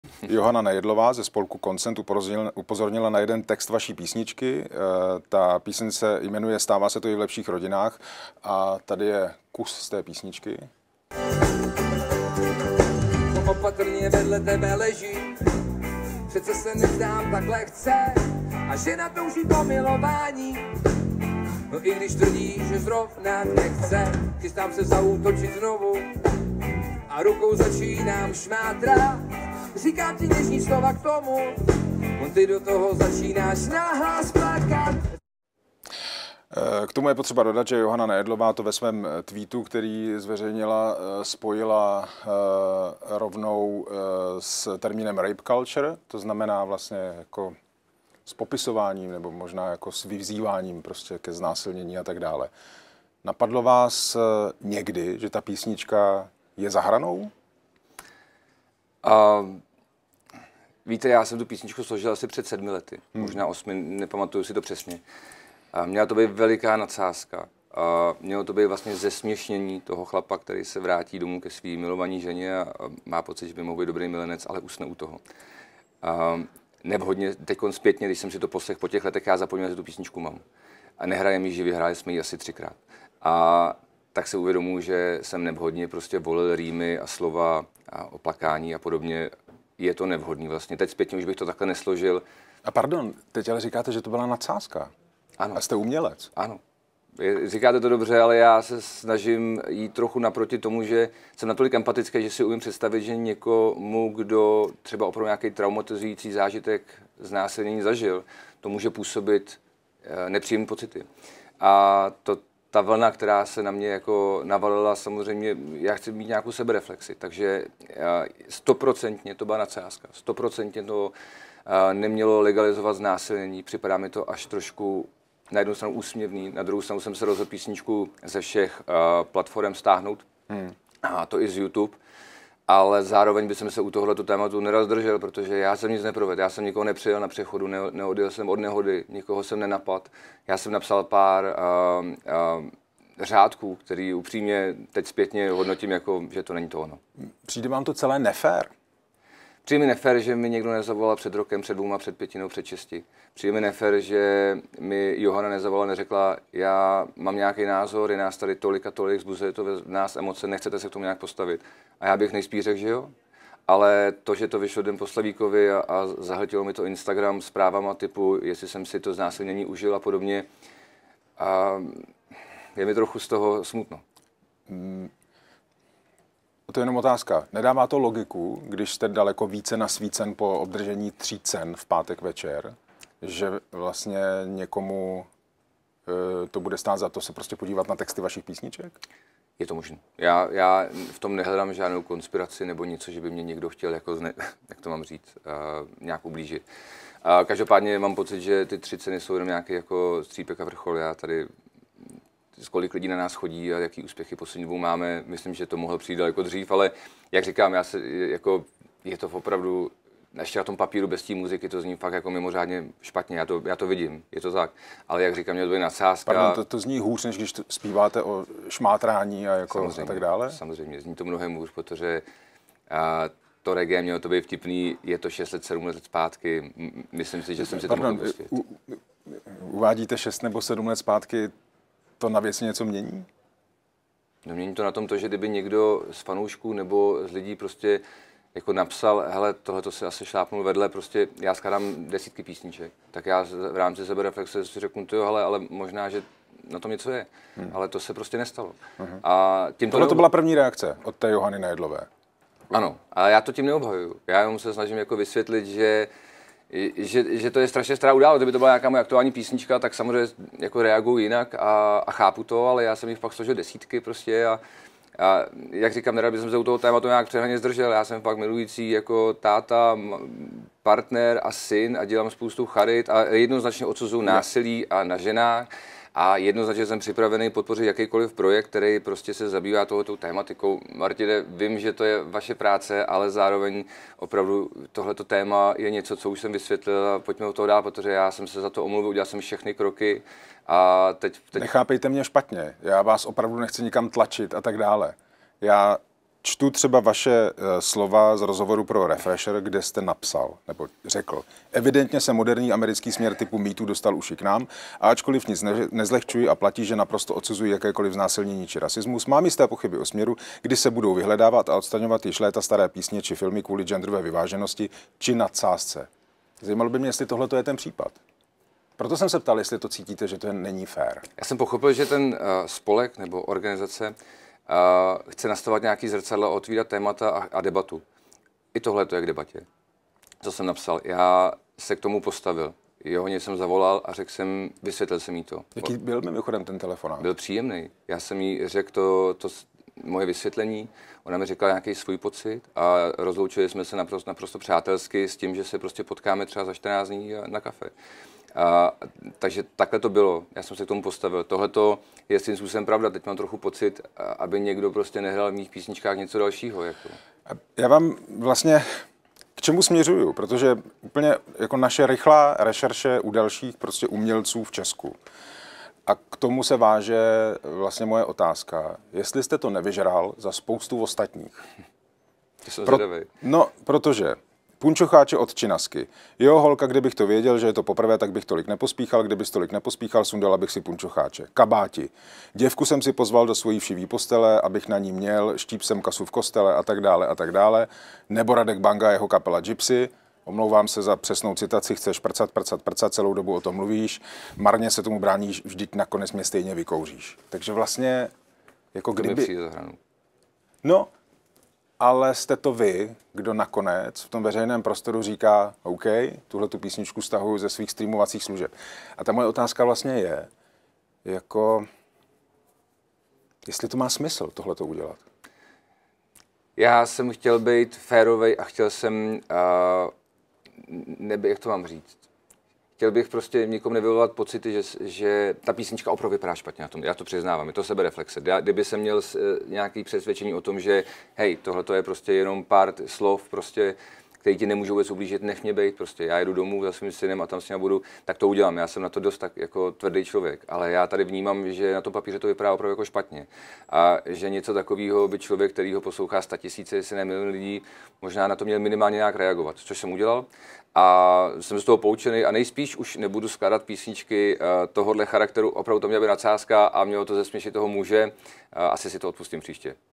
Ztracený: Jsem zlomenej, nikomu jsem neublížil, píseň byla nadsázka. Janečkův projev byl nevkusný
Jsem plný emocí, ceny mám díky fanouškům. Zažil jsem plno úspěchů a pádů, nikomu jsem nikdy nechtěl ublížit. Sedm let stará písnička je nadsázka, dnes už bych ji takto nenapsal. Postavil jsem se k tomu, chci být férový, písničku jsem stáhl. Jednoznačně odsuzuji násilí na ženách, říká zpěvák Marek Ztracený, který se stal absolutním vítězem v obnovené hudební anketě Český slavík 21. Mně se vždy líbilo, že Slavík vyvolává emoce, ale toto už je přehnané, řeči Řezníka i Janečka byly nevkusné, připadal jsem si jako na předvolební kampani, dodává.